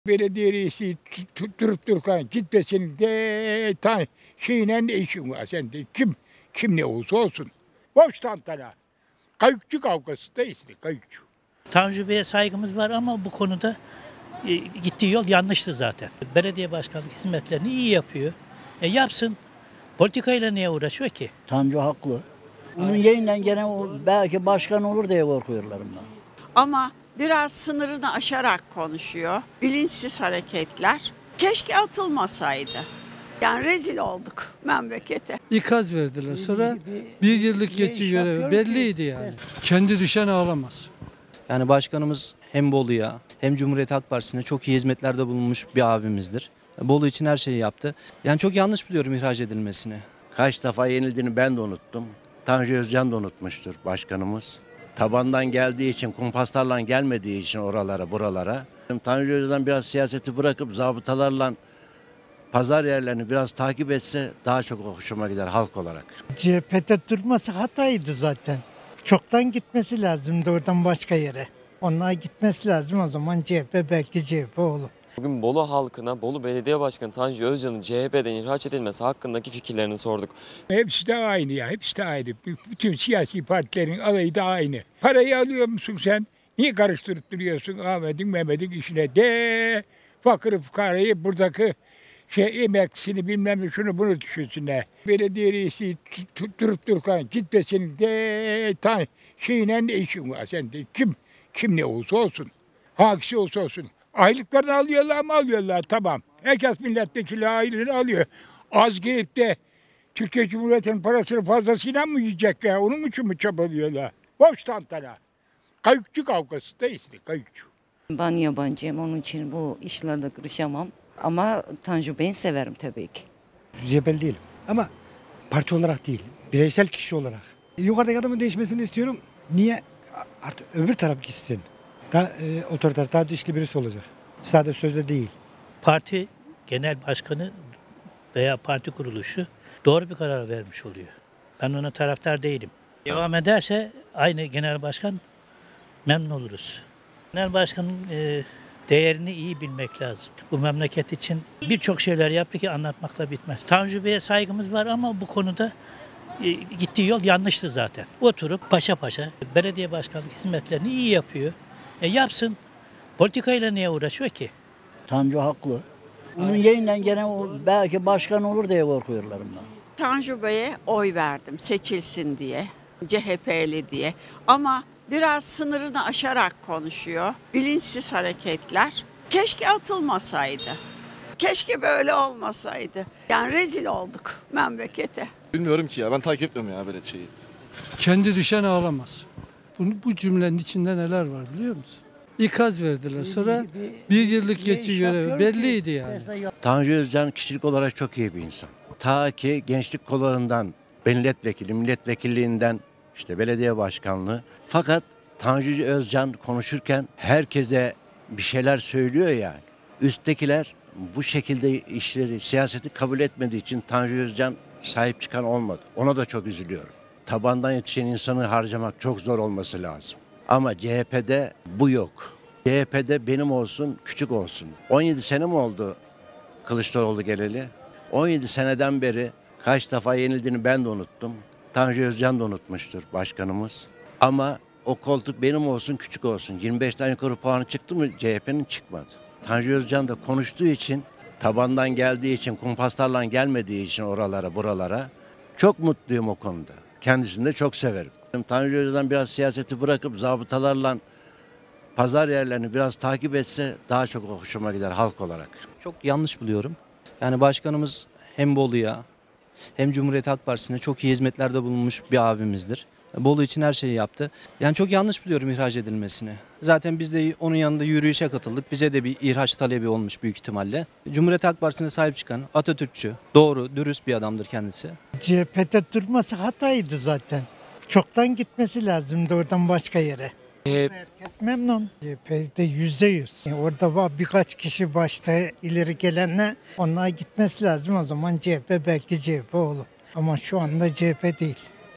Tanju Özcan'ın CHP'den ihraç kararı vatandaşı ikiye böldü! | Sokak Röportajı
Tanju Özcan'ın CHP'den ihraç edilme kararının ardından yapıtığımız sokak röportajında,  Özcan'ın ihraç süreci ve bu durumun Türkiye siyaseti üzerindeki etkisini vatandaşların gözünden inceliyoruz. Özcan'ın ihraç kararıyla ilgili vatandaş görüşleri ve yorumlar için videomuzu izlemeye devam edin.